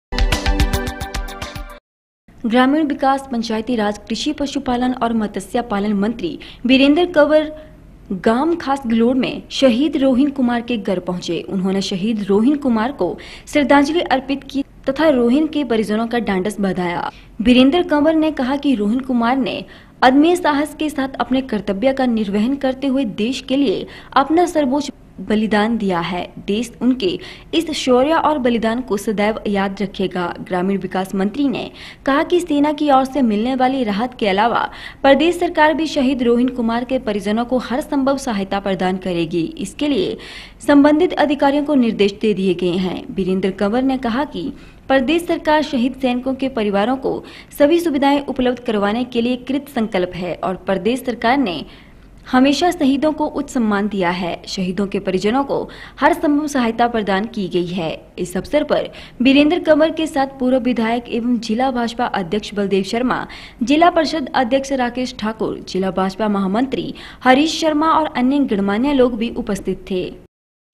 3 AUG 2 शहीद रोहिन कुमार के परिजनों से मिले ग्रामीण विकास, पंचायतीराज एवं कृषि मंत्री वीरेंद्र कंवर
ग्रामीण विकास, पंचायतीराज, कृषि, पशुपालन और मत्स्य पालन मंत्री वीरेंद्र कंवर गांव गलोड़ खास में शहीद रोहिन कुमार के घर पहुंचे। उन्होंने शहीद रोहिन कुमार को श्रद्धांजलि अर्पित की तथा रोहिन के परिजनों को ढांढस बंधाया।